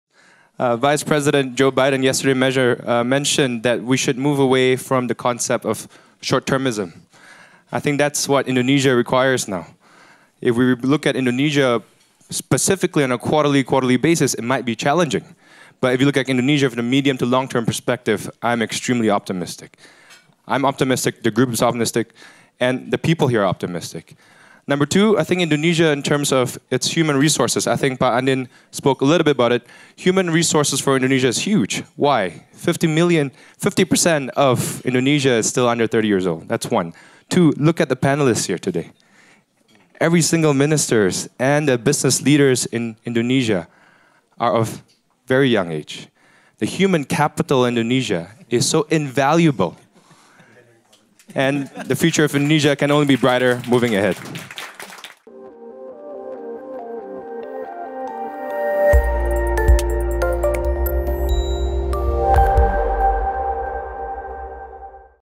Anderson Tanoto at Indonesia Luncheon during World Economic Forum 2016 at Davos, Switzerland
Anderson Tanoto gives his comments on Indonesia's future growth, closing the Indonesia Lunch Dialogue at World Economic Forum in Davos, Switzerland, on 21 January 2016.

The dialogue focused on the challenges and opportunities for Indonesia in view of global and regional economic growth, domestic political reform, and security. The esteemed panel included Indonesia Trade Minister Tom Lembong, Indonesia Finance Minsiter Bambang Brodjonegoro, Bank Mandiri CEO Budi Sadikin, Lee Kuan Yew School of Public Policy Dean Professor Kishore Mahbubani, and Indonesia Chamber of Commerce (KADIN) Vice-Chairman Anindya Bakrie.

Mr Tanoto is an RGE Director and APRIL Group shareholder.